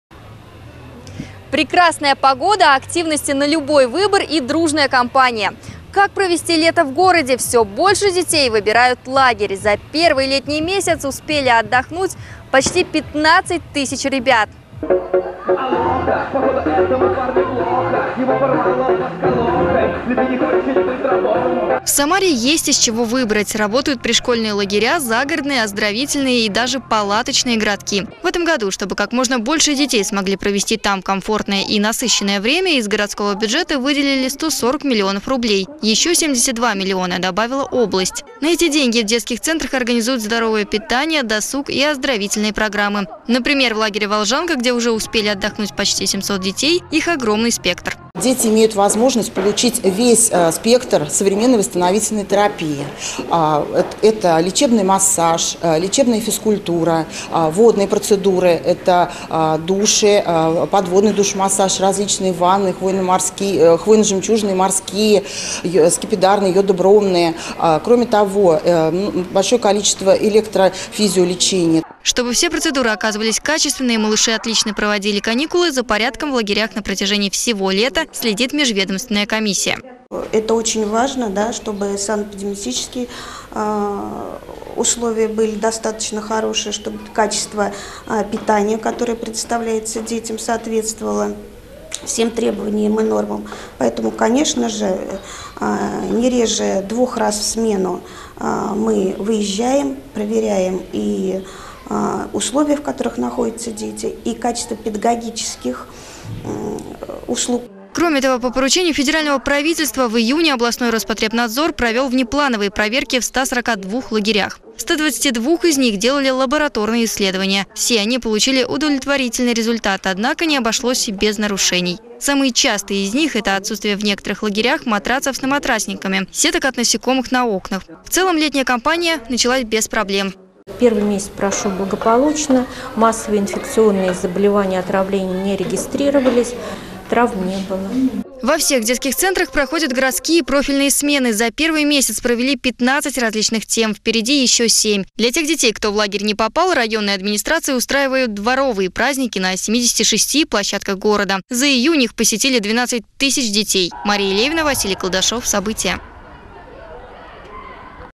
Работают пришкольные лагеря, загородные, оздоровительные и даже палаточные городки
Лето в самом разгаре. Тысячи детей уже успели отдохнуть в городских лагерях.  В каждом из них проводят Профильные смены. На протяжении всей летней Кампании  Межведомственная комиссия следит за комфортом и безопасностью в них. Сегодня обсудили итоги Первого месяца. Мария Левина расскажет подробнее.